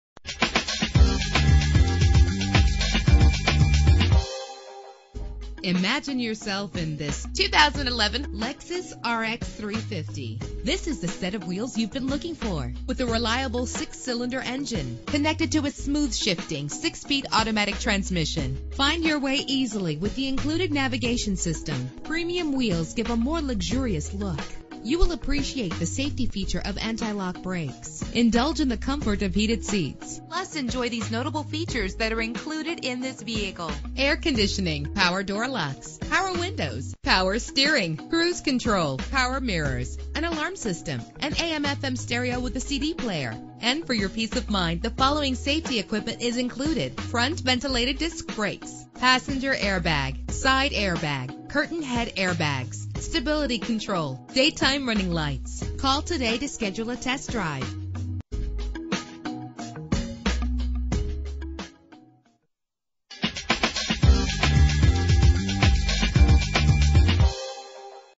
2011 Lexus RX 350 - Viera FL
Year: 2011
Make: Lexus
Model: RX 350
Trim: NAV
Engine: 3.5 liter 6 cylinder 24 valve
Transmission: 6-Speed Automatic
Color: Black
Mileage: 10
Address: